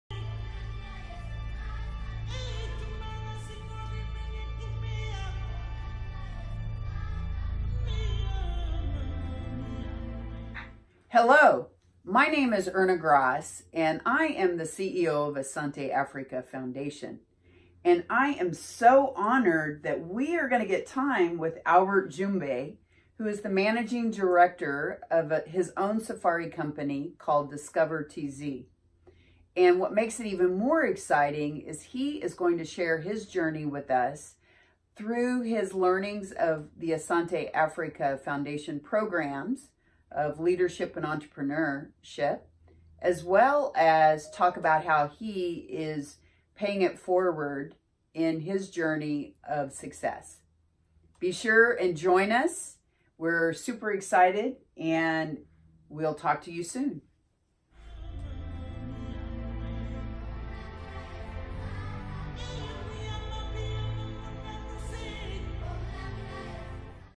Albert Jumbe's interview on his journey from Education to Entrepreneurship - Teaser
Erna Grasz interviews Albert Jumbe, Asante Africa Scholarship Coordinator and DiscoverTz Safari Company Owner, on his journey from Education to Entrepreneurship. Full Video Coming soon.